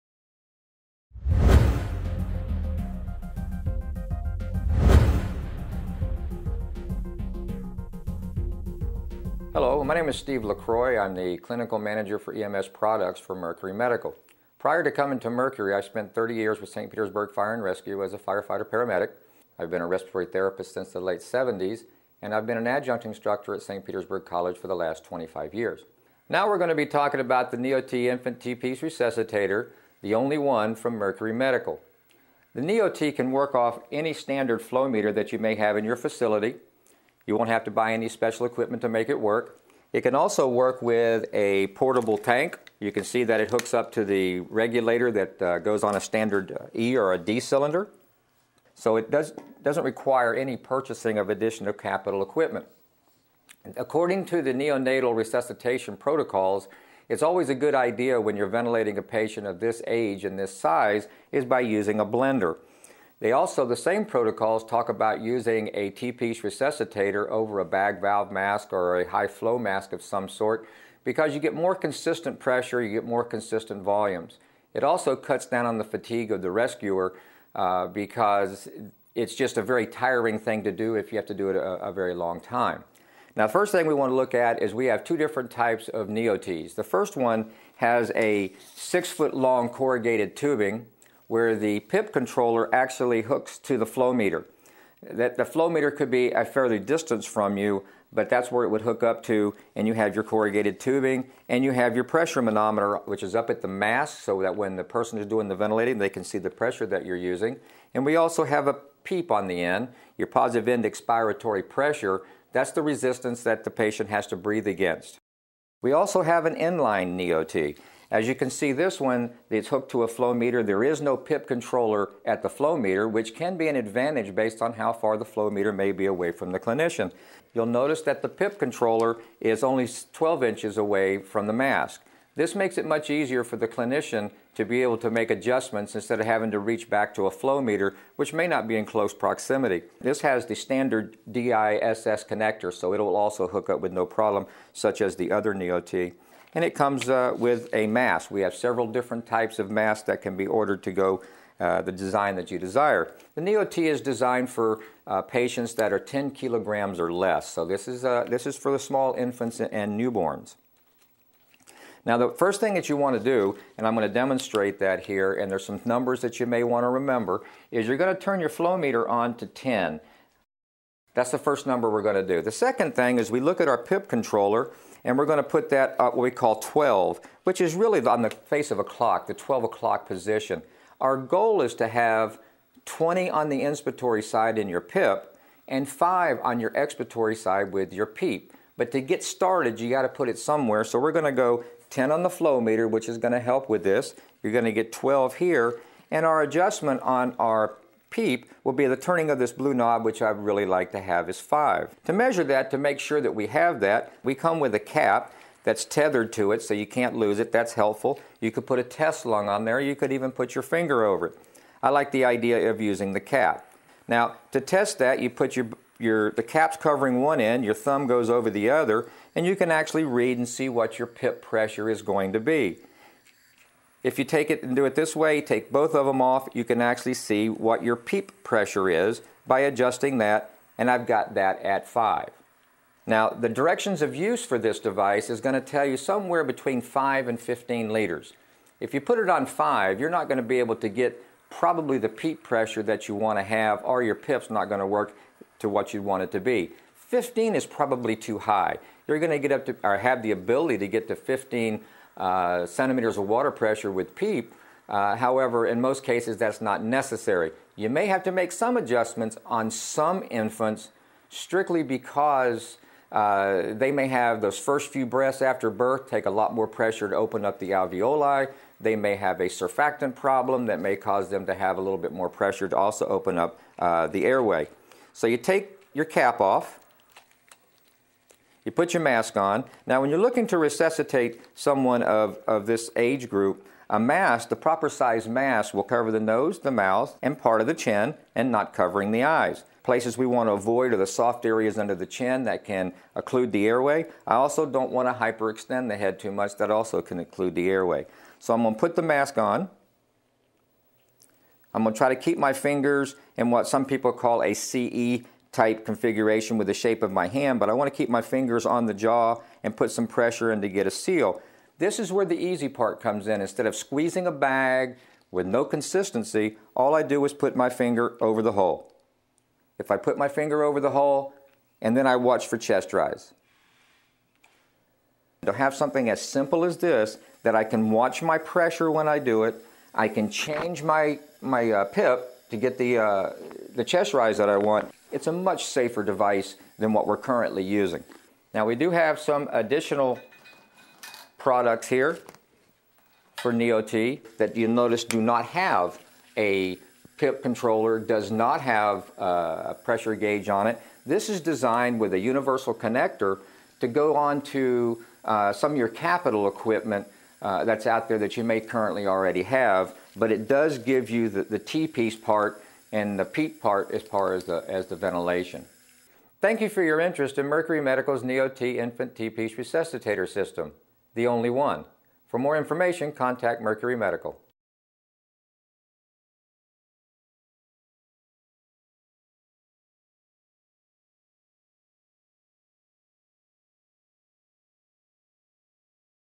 Neo Tee
Dispositivo para adminstración controlada de respiración en Neonatos, directamente desde la fuente de O2, sin necesidad de Flujómetro.